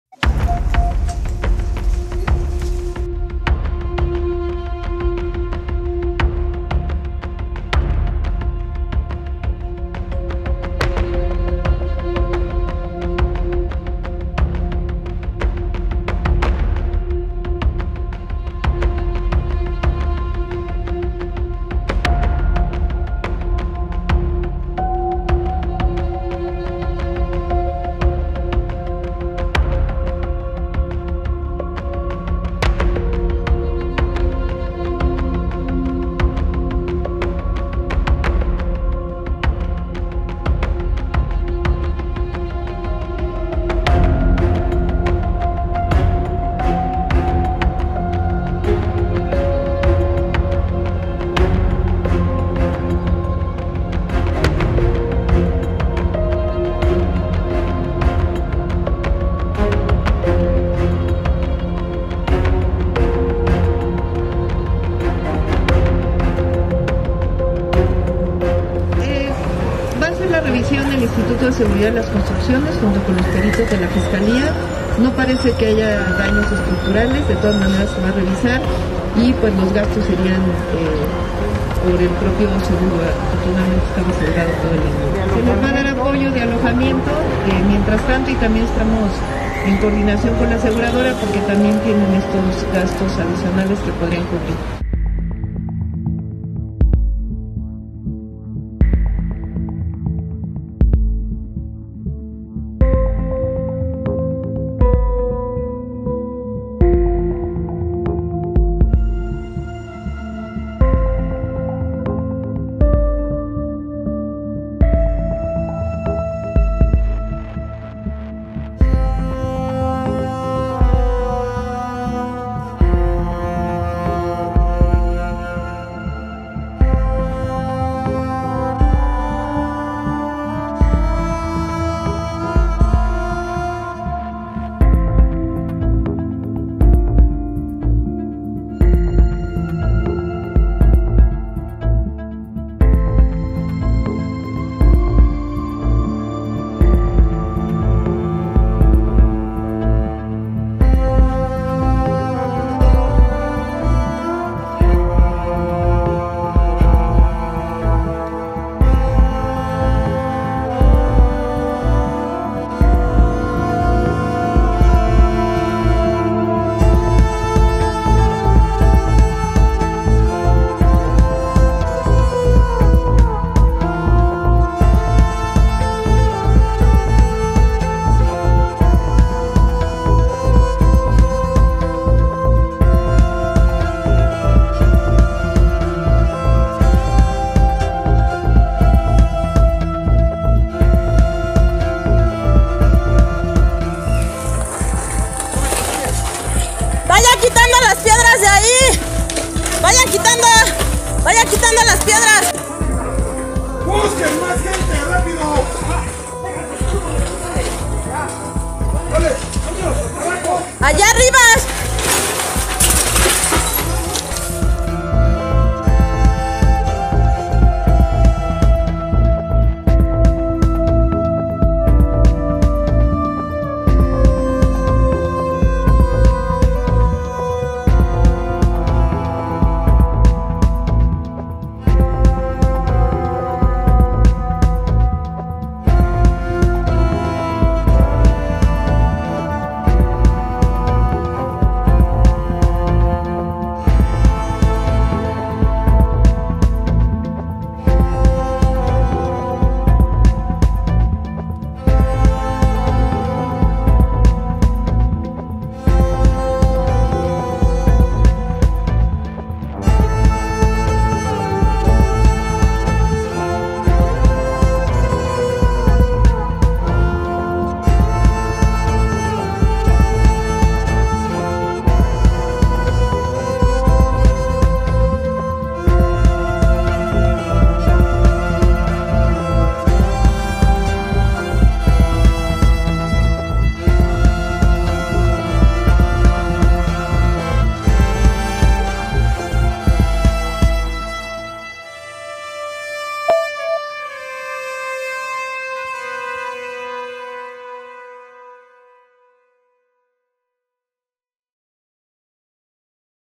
Explosión de gas en edificio de Coyoacán en CDMX deja 26 heridos
La acumulación de gas en un departamento de Av. Coyoacán en la Alcaldía Benito Juárez de la CDMX ocasionó un estallido que se cobró la vida de una persona y más de 20 personas resultaron heridas.